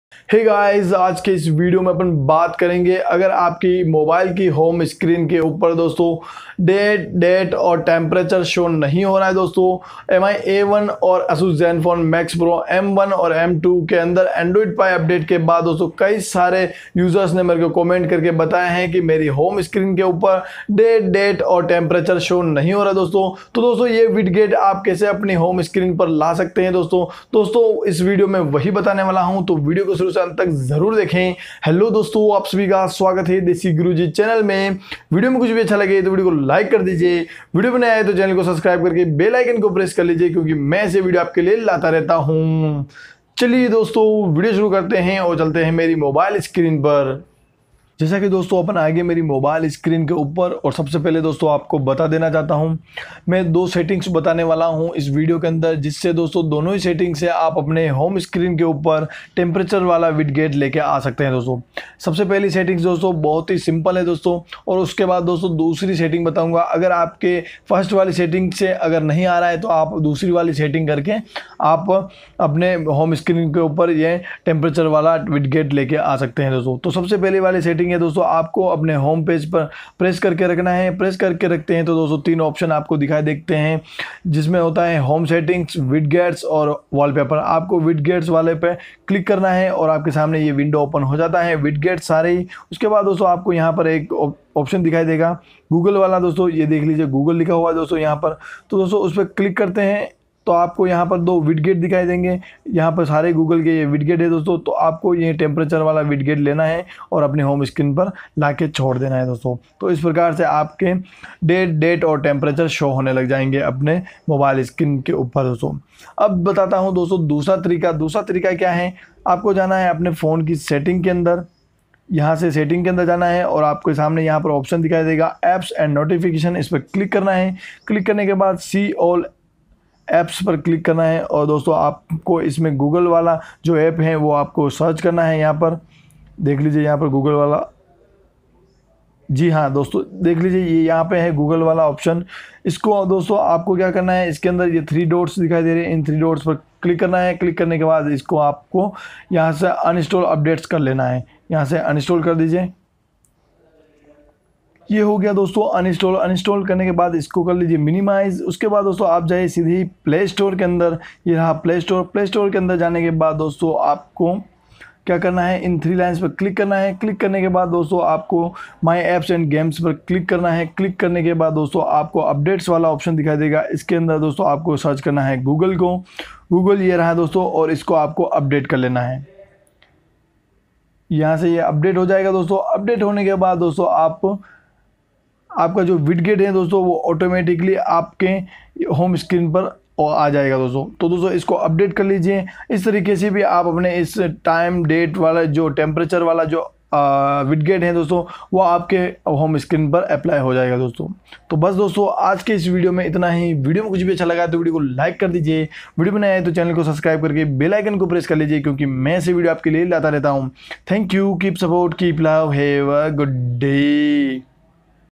Mi A1 How to Show Temperature on Home Screen | Asus Max Pro M1 | Zenfone Pro M2 | Desi Guruji
Hello Dosto

Aj apn bat karege ap abhi phone ko home screen par kaise day date or temperature kaise show Kar Skte ho.